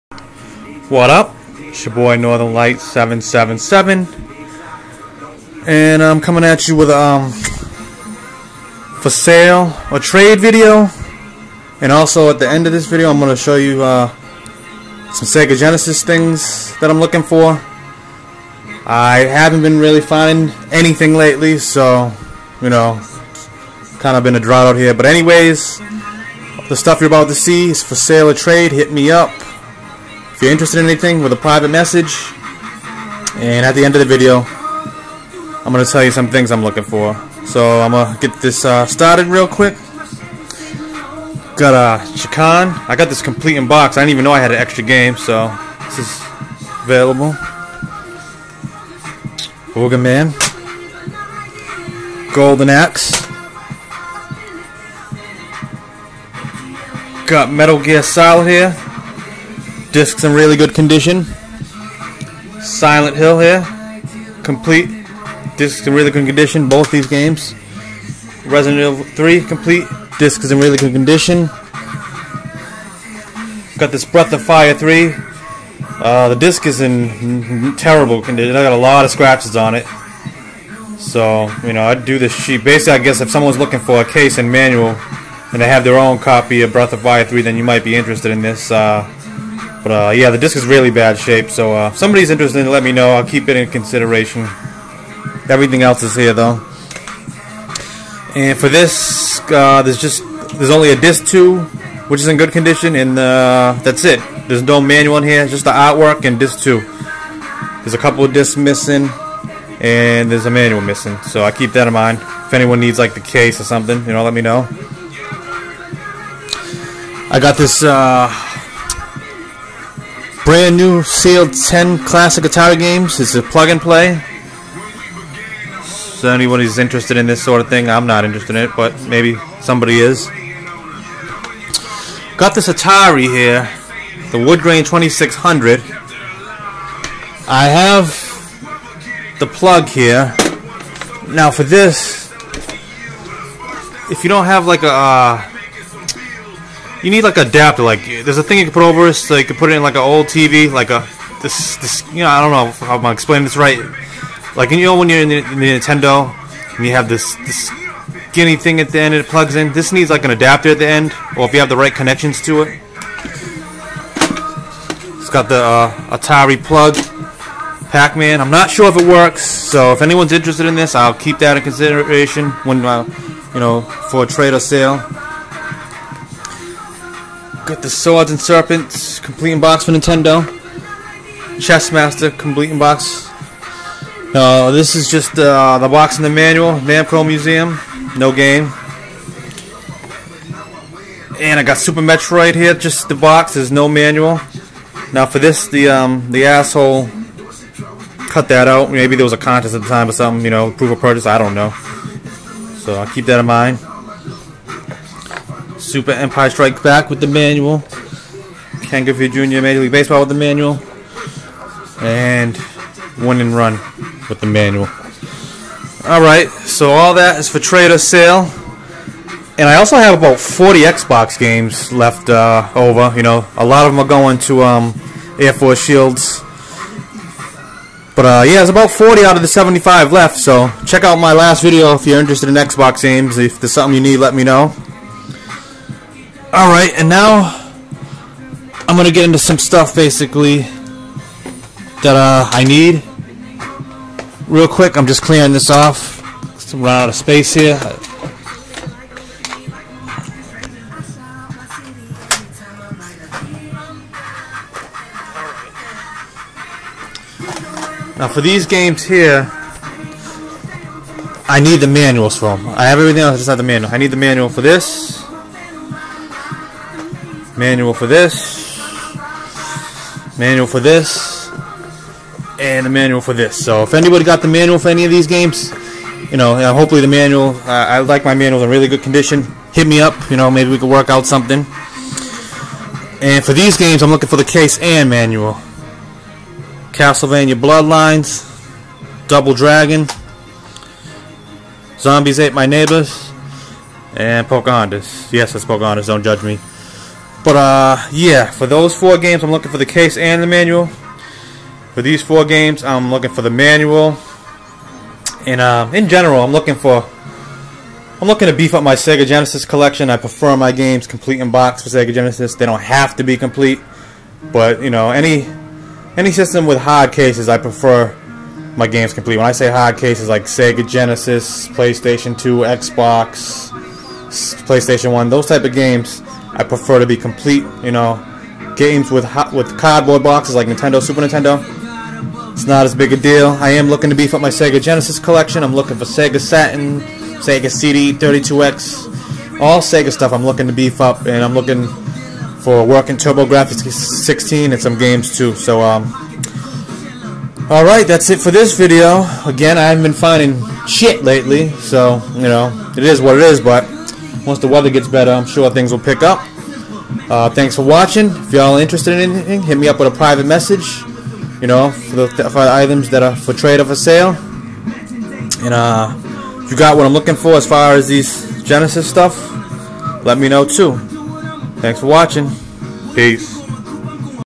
Items for Trade or Sale and some Sega Genesis Items im looking for.......
A bunch of different items for trade or sale from Nintendo, Super Nintendo, PS1, Genesis, Atari and more! Also some Genesis items im looking for! Im also looking for any Sega console games as well as a working Turbo Grafx 16 and games.